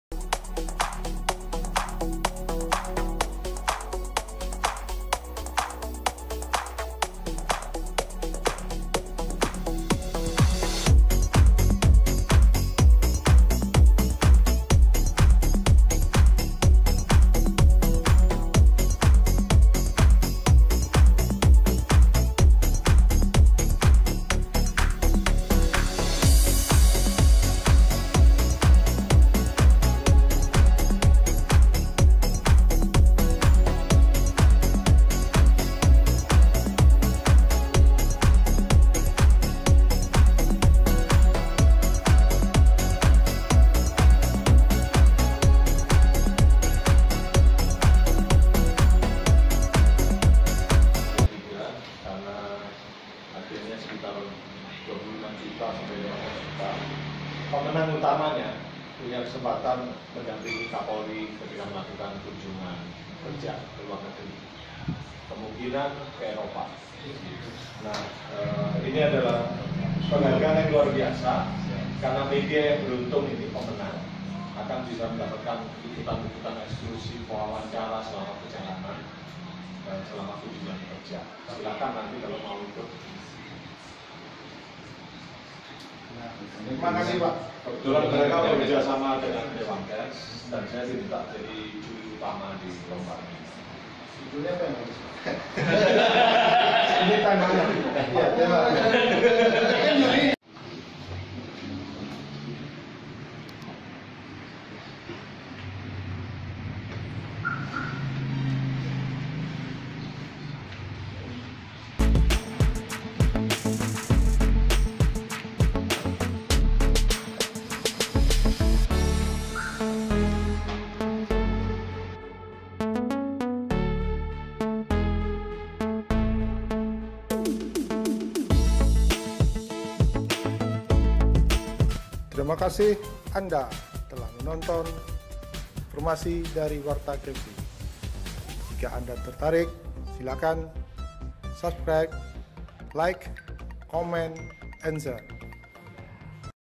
File Ketua Dewan Pers Yosep Adi Prasetyo Verifikasi Faktual Media WartaKepri.co.id Tahun 2018
WartaKepri TV Ketua Dewan Pers Yosep Adi Prasetyo Verifikasi Faktual Media WartaKepri.co.id Tahun 2018